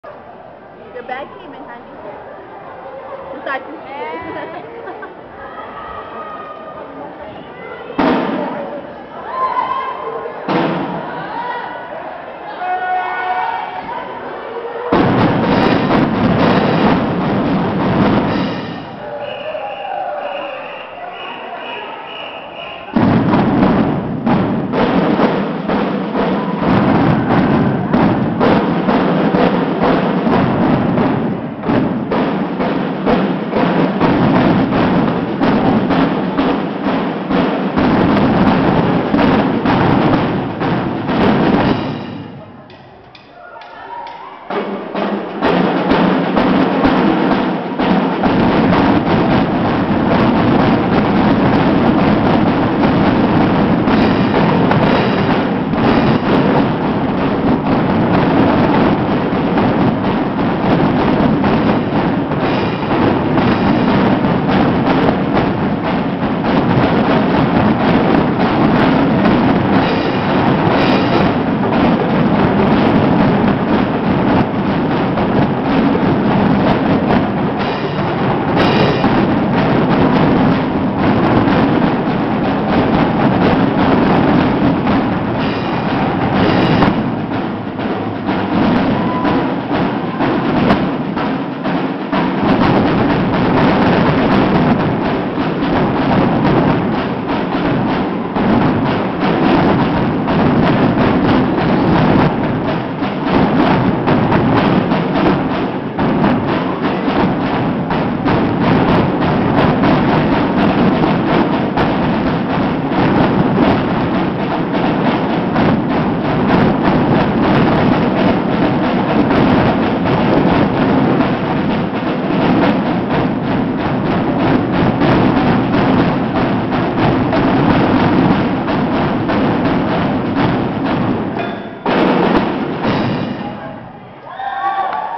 MHS JROTC
MHS JROTC  performing at the culutre festival 2010.